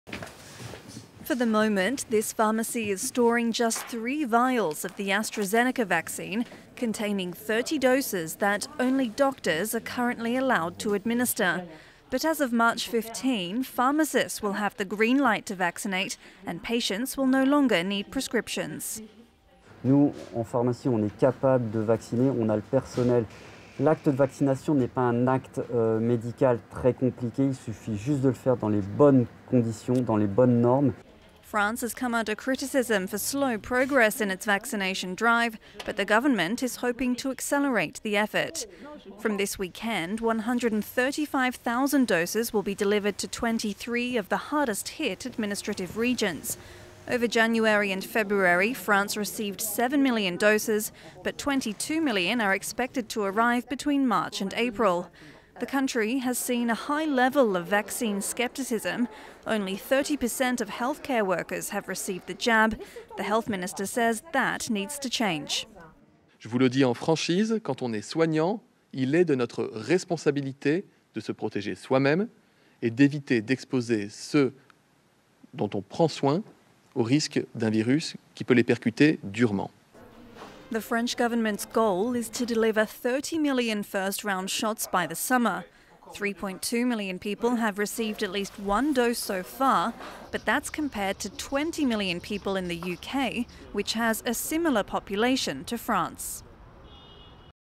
France aims to vaccinate 30 million against Covid-19 by the summer
France aims to have vaccinated at least 10 million people by mid-April, 20 million by mid-May and a total of 30 million by the summer, Prime Minister Jean Castex said on Thursday.